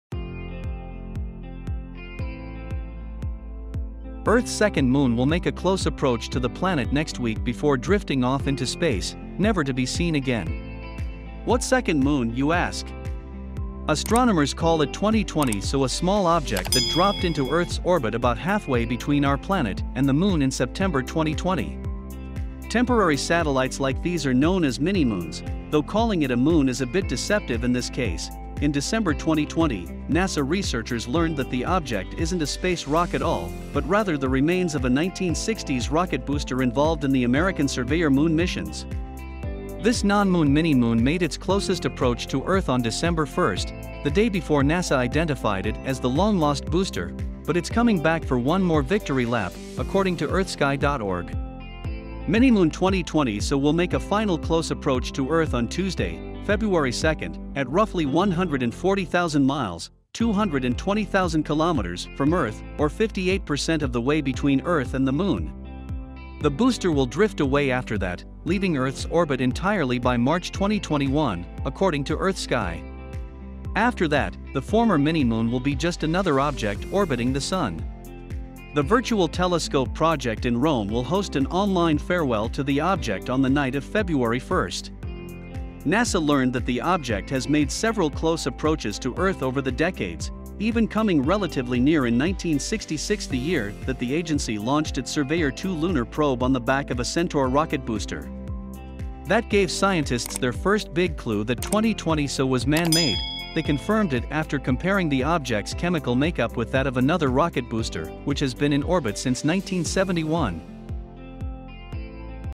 Earth is about to lose its second moon, forever@globalnews
Earth's second moon will make a close approach to the planet next week before drifting off into space, never to be seen again.

"What second moon," you ask? Astronomers call it 2020 SO — a small object that dropped into Earth's orbit about halfway between our planet and the moon in September 2020. Temporary satellites like these are known as minimoons, though calling it a moon is a bit deceptive in this case; in December 2020, NASA researchers learned that the object isn't a space rock at all, but rather the remains of a 1960s rocket booster involved in the American Surveyor moon missions.

This non-moon minimoon made its closest approach to Earth on Dec. 1 (the day before NASA identified it as the long-lost booster), but it's coming back for one more victory lap, according to EarthSky.org. Minimoon 2020 SO will make a final close approach to Earth on Tuesday (Feb. 2) at roughly 140,000 miles (220,000 kilometers) from Earth, or 58% of the way between Earth and the moon.
The booster will drift away after that, leaving Earth's orbit entirely by March 2021, according to EarthSky. After that, the former minimoon will be just another object orbiting the sun. The Virtual Telescope Project in Rome will host an online farewell to the object on the night of Feb. 1.
NASA learned that the object has made several close approaches to Earth over the decades, even coming relatively near in 1966 — the year that the agency launched its Surveyor 2 lunar probe on the back of a Centaur rocket booster. That gave scientists their first big clue that 2020 SO was man-made; they confirmed it after comparing the object's chemical makeup with that of another rocket booster, which has been in orbit since 1971
Earth is about to lose its second moon, forever

The free info channel is the best place to watch documentaries